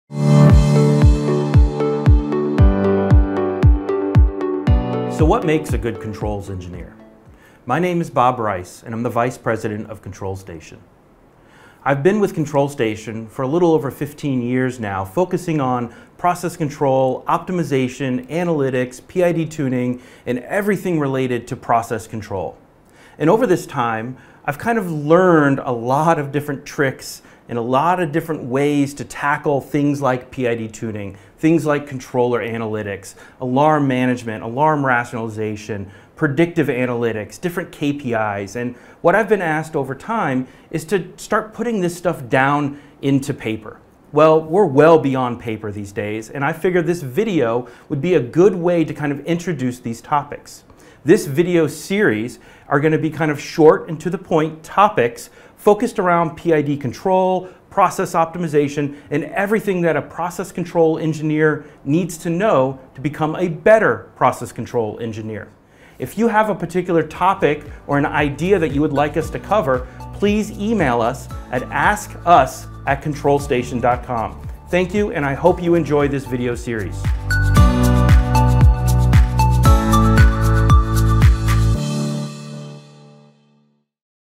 Ask Dr Bob: An Introduction
Struggling with PID control? Looking for best practices? Need simple, understandable answers? Just Ask Bob.

Our next video series will feature Bob Rice. He'll provide no-nonsense answers to common Process Control and plant optimization challenges.